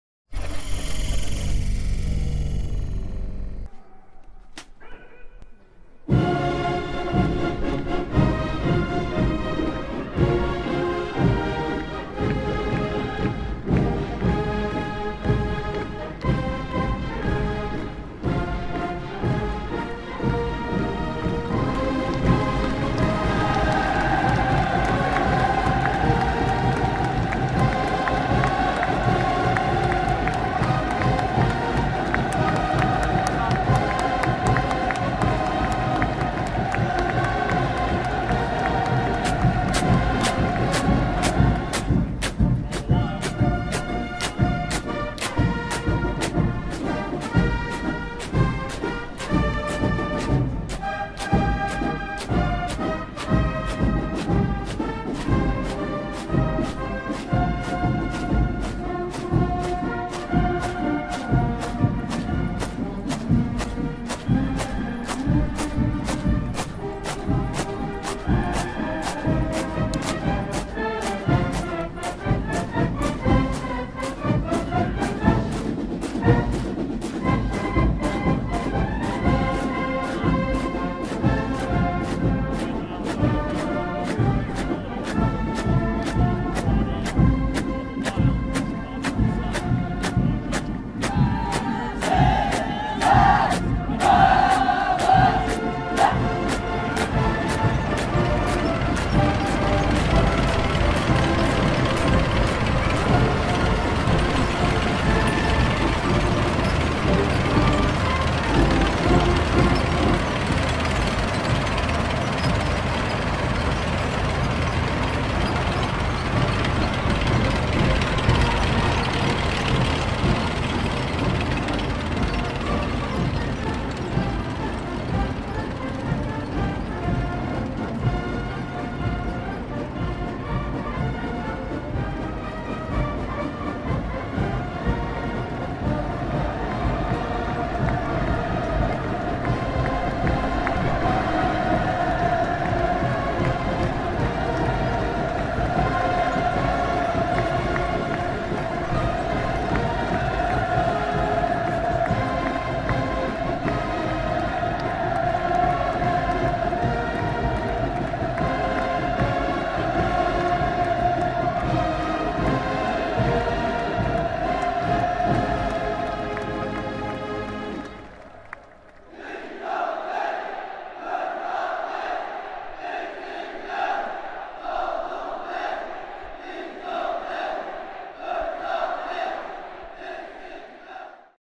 Parade - North Korea - First Nuclear Missile - FULL HD worldarmy North Korea
Parade - North Korea - First Nuclear Missile - FULL HD
North Korea's first military parade with nuclear missiles. The appearance of two North Korean leaders, Kim Jong-In and Kim Jong-Un